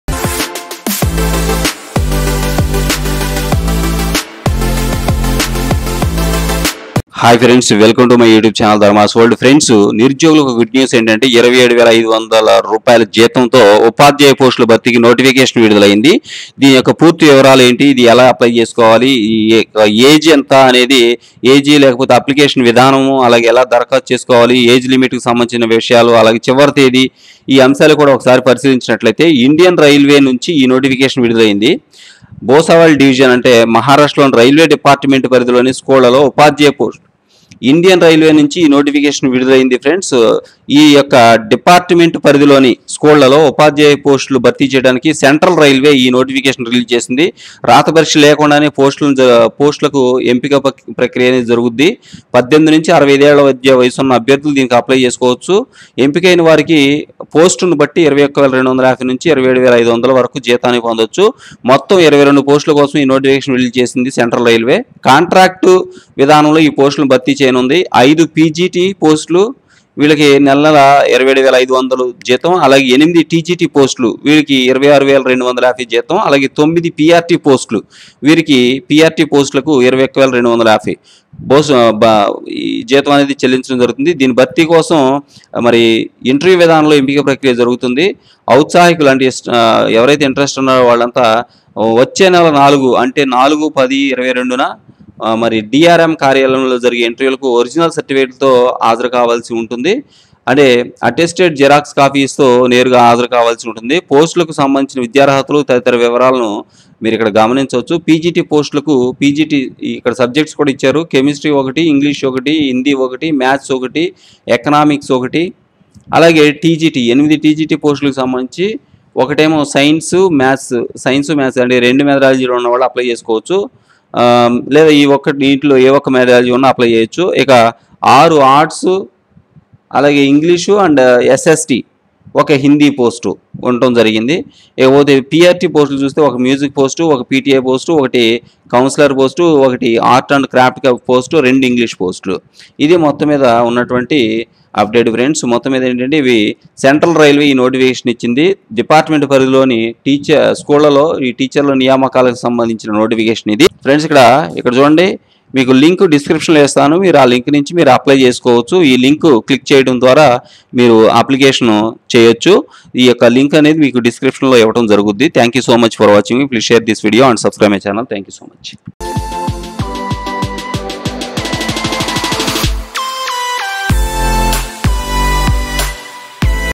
నిరుద్యోగులకు గుడ్ న్యూస్ రూ 27,500 జీతంతో ఉపాధ్యాయ పోస్టుల భర్తీకి నోటిఫికేషన్ విడుదలFULL DETAILS
ధన్యవాదాలు